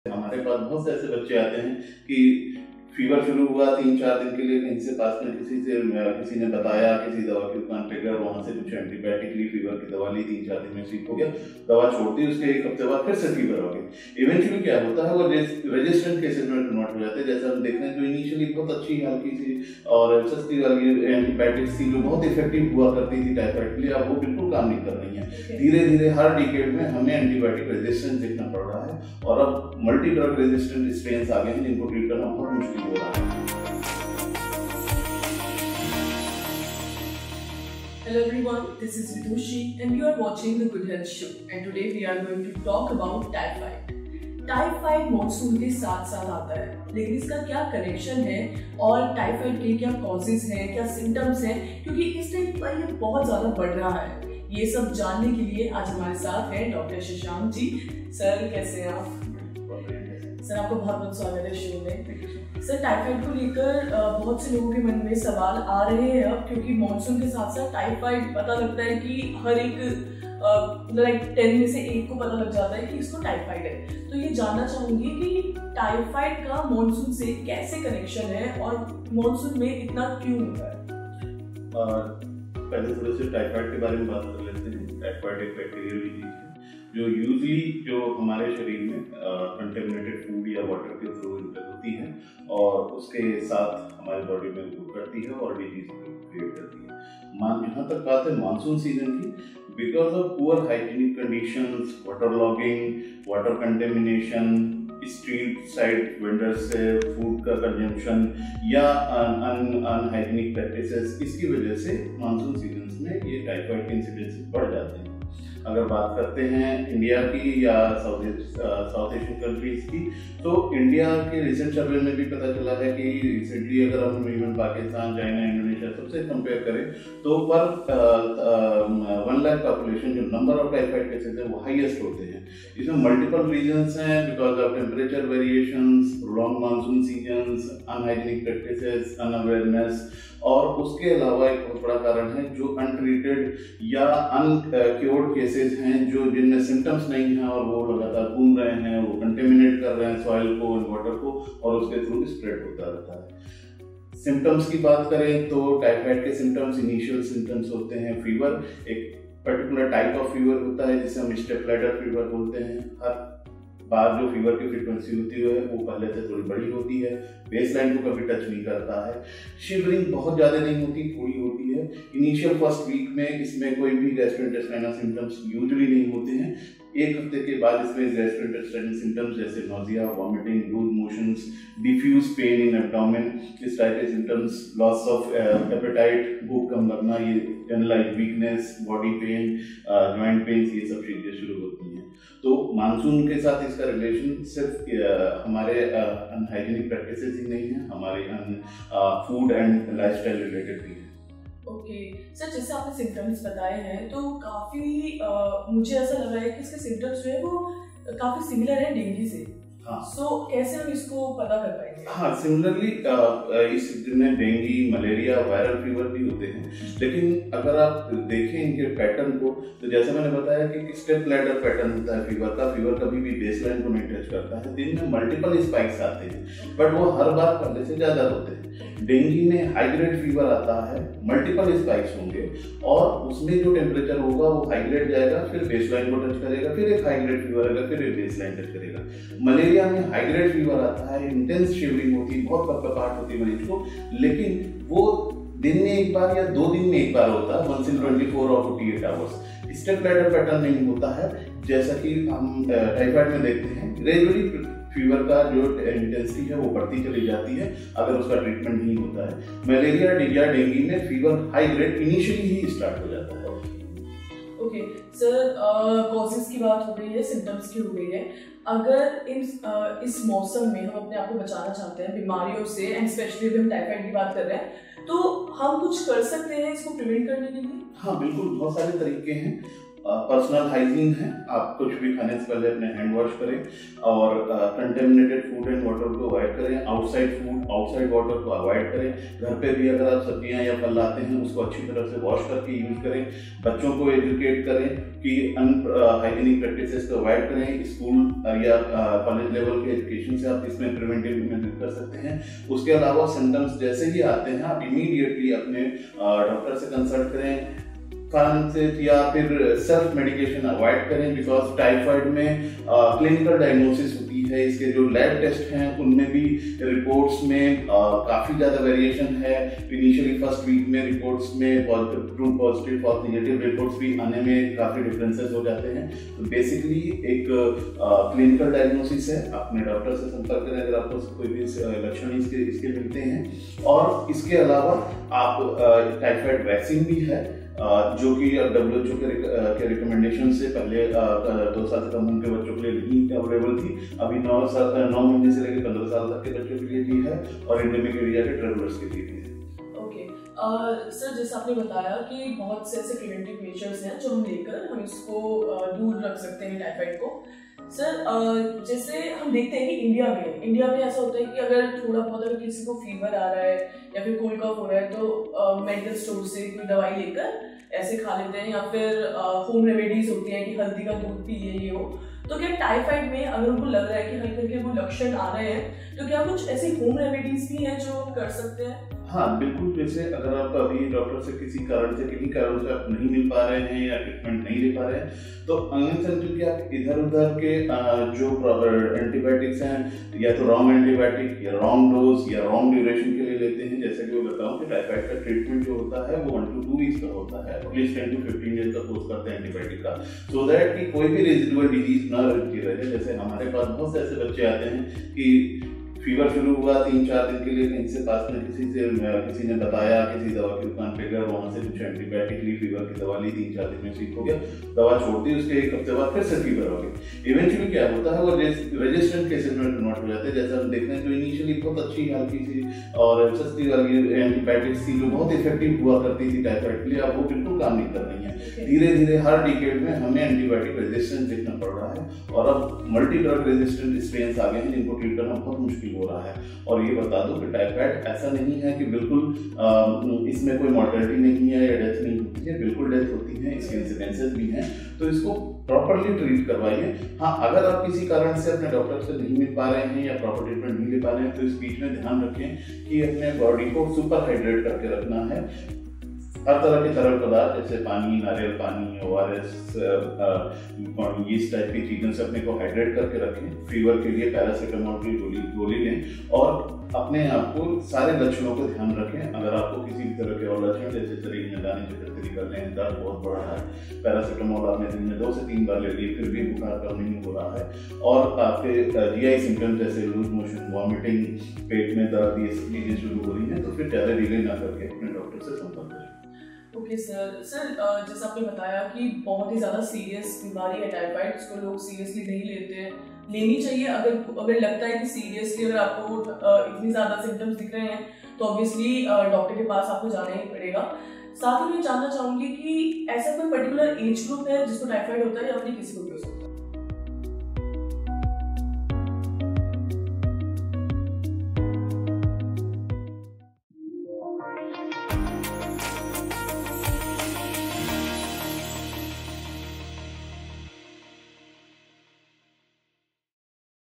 Typhoid Fever: Symptoms, Diagnosis, Treatment, Vaccine | Dr Shashank | TGHS | Healthians | Ep182
Join us for an exclusive interview with renowned infectious disease specialist, Dr. Shashank, as our host Vidushi delves into the world of Typhoid. In this informative discussion, we explore the causes, symptoms, prevention, and treatment of TyphoidFever, a serious infectious disease. Dr. Shashank, a respected MedicalExpert in the field of InfectiousDiseases, shares his extensive knowledge and insights.

Discover how to safeguard your health and stay informed about the latest developments in typhoid awareness. This in-depth conversation is a valuable resource for HealthEducation, helping you understand the importance of early detection and prevention.

Don't miss out on this engaging episode of TheGoodHealthShow, presented by Healthians. Together, let's prioritize our health and well-being. 🩺💡